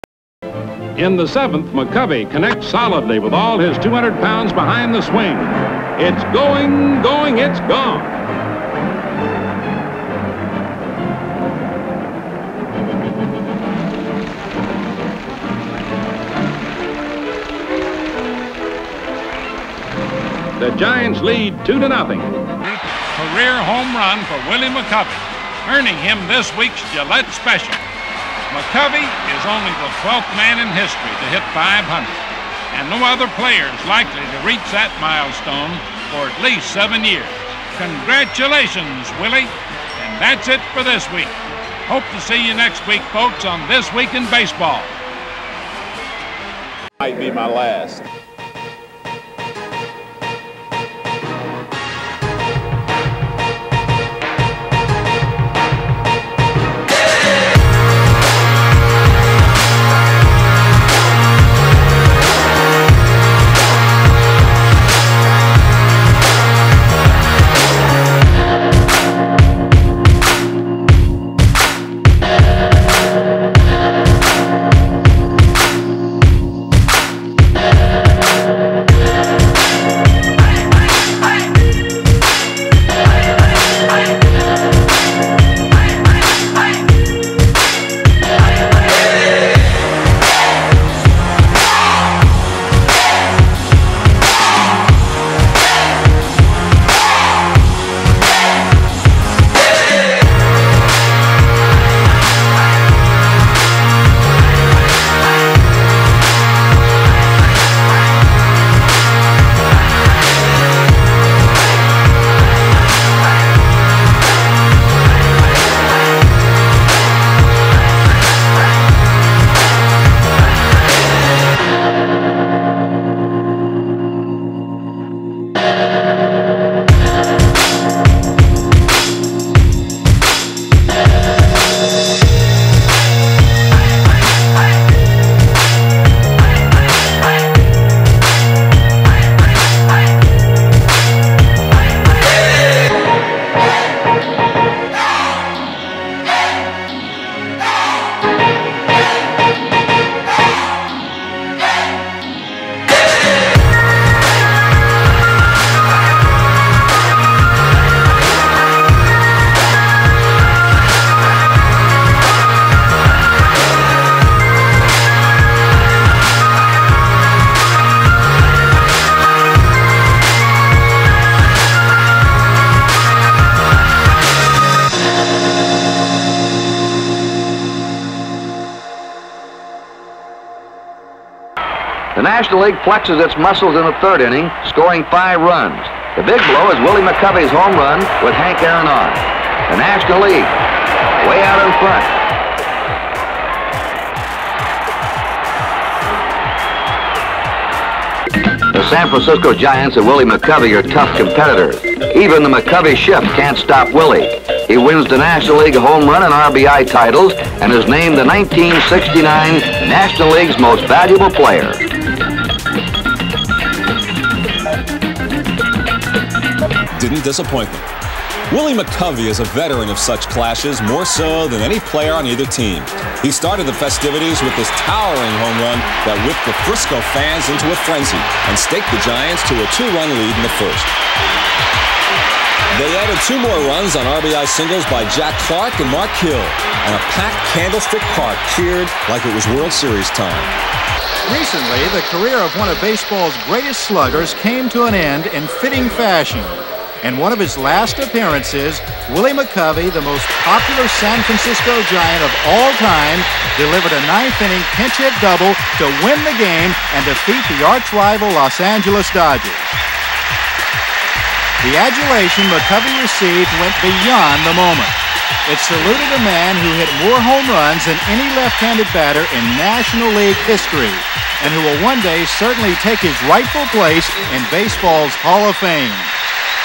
Willie McCovey MASHING home runs | The 1969 MVP had INSANE power!
Willie McCovey was one of the most feared hitters EVER.

To celebrate Black History Month, MLB Vault will take a look back at some former MVPs.